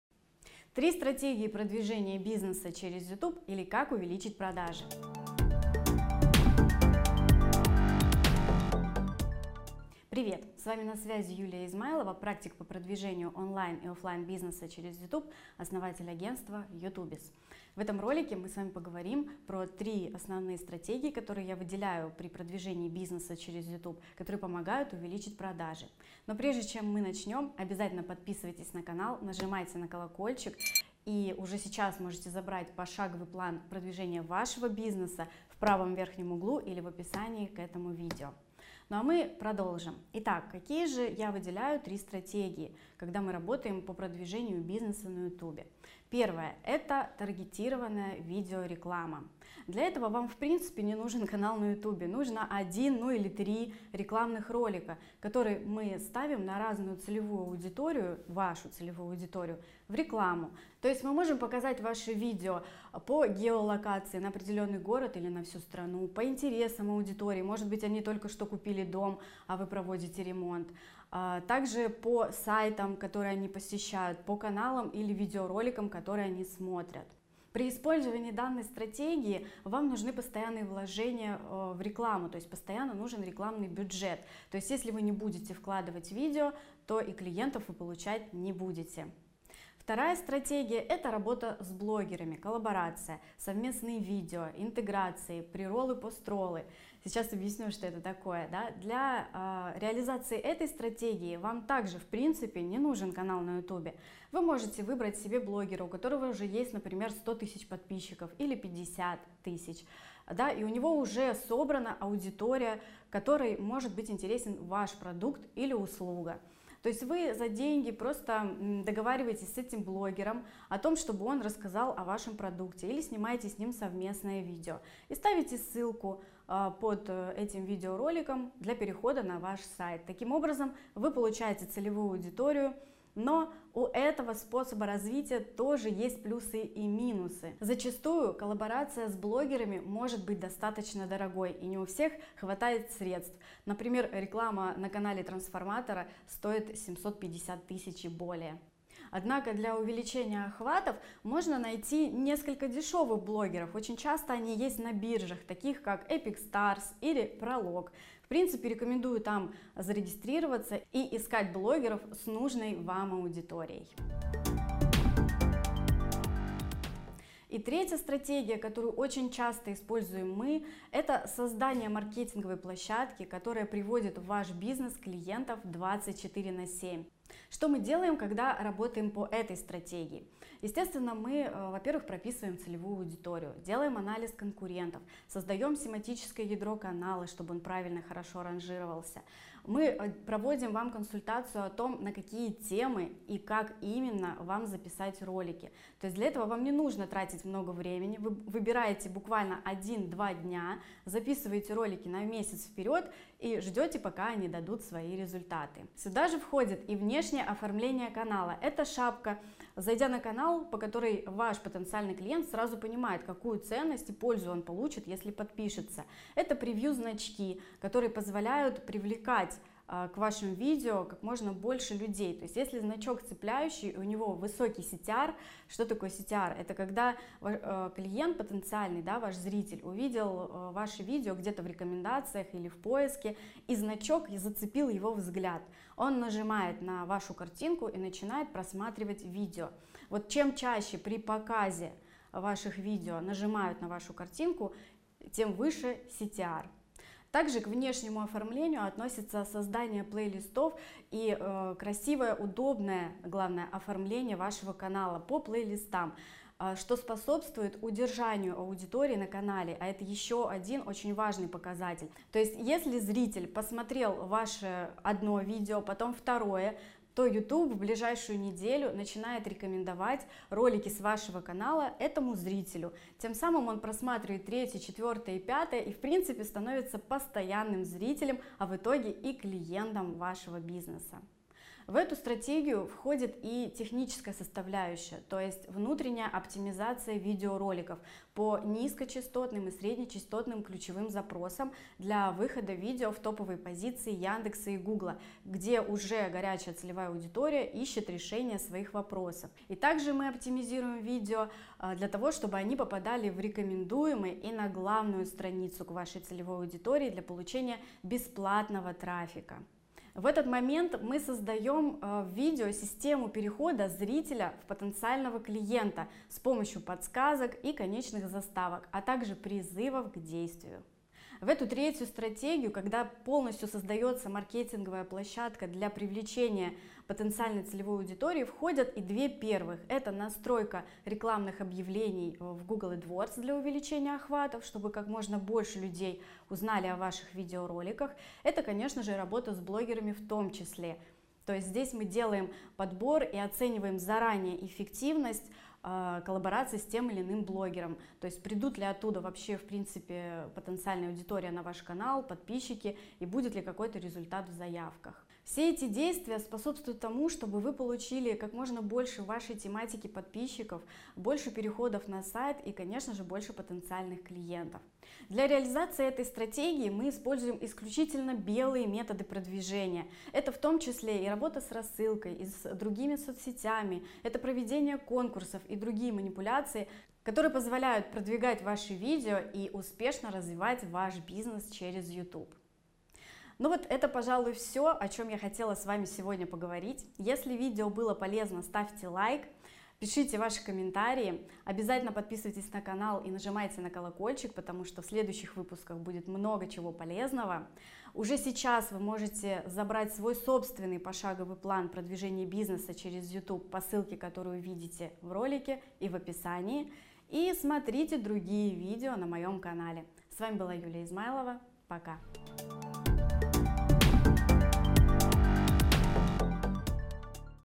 3 стратегии продвижения бизнеса через Youtube или как увеличить продажи 18+
3 стратегии продвижения бизнеса через Youtube или как увеличить продажи?
Запишитесь на бесплатный аудит канала или Узнайте Пошаговый план продвижения своего бизнеса через YouTube, ответив всего на 7 вопросов.
В этом ролике мы поговорим про три основные стратегии, которые я выделяю при продвижении бизнеса через ютуб, которые помогают увеличить продажи.
0:20 Как увеличить продажи с помощью видео на YouTube?
0:46 Таргетированная видеореклама
1:30 Работа с блогерами
2:48 Создание маркетинговой площадки
Напишите в комментариях, какими методами продвижения своего бизнеса на ютубе пользуетесь вы?